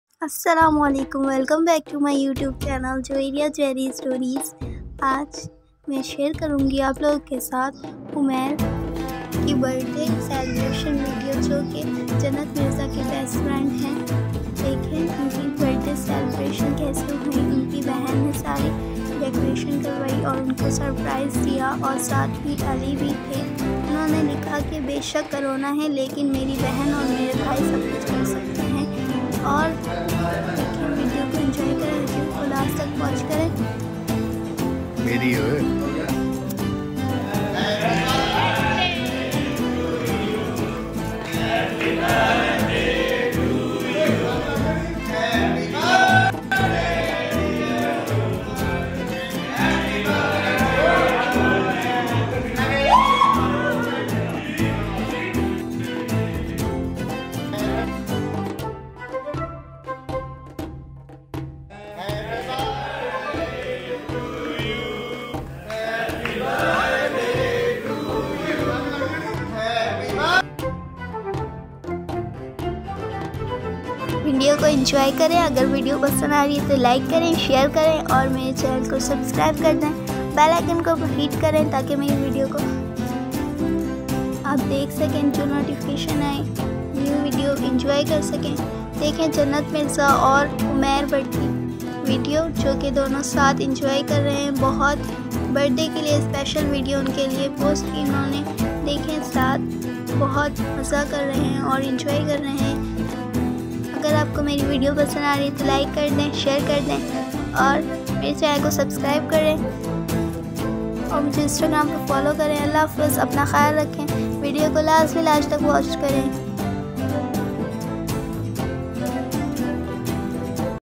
jannat mirza ky Best friend Umer Butt Birthday celebration with family and friends
Hi guys ☺️

javeria jerry stories

mein share krou gi jannat mirza ky mgetar or best friend umer butt ki birthday celebration with mama and family friend surprised her sister and vedio lazmi watch krynn..
Comments